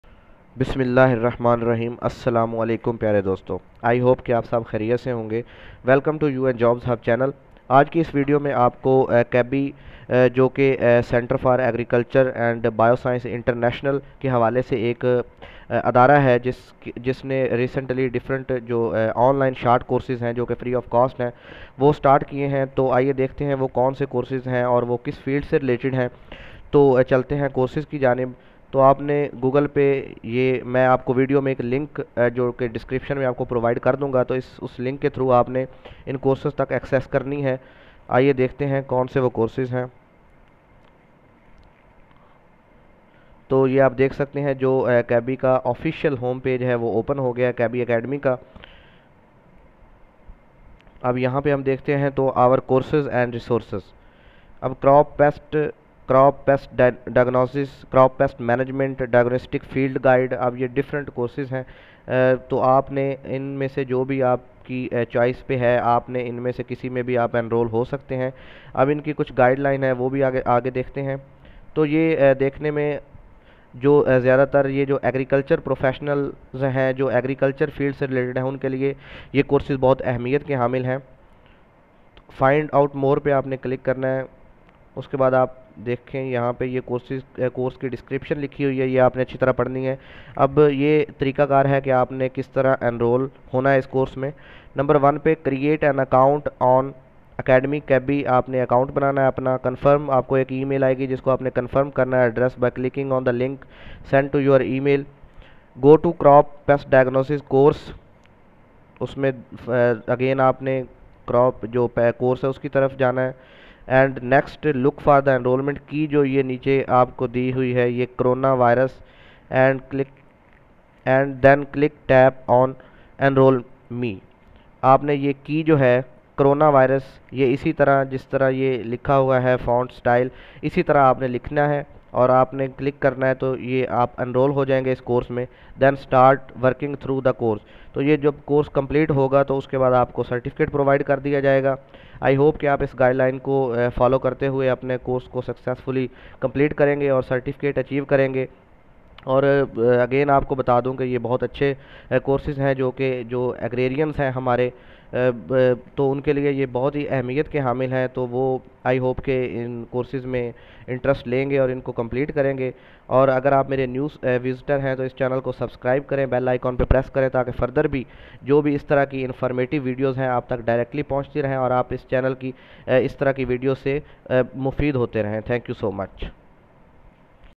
Free Online Short Courses from CABI
Detailed Information regarding Free Short Courses of CABI (Center for Agriculture and Bioscience International)
Link for Enrollment: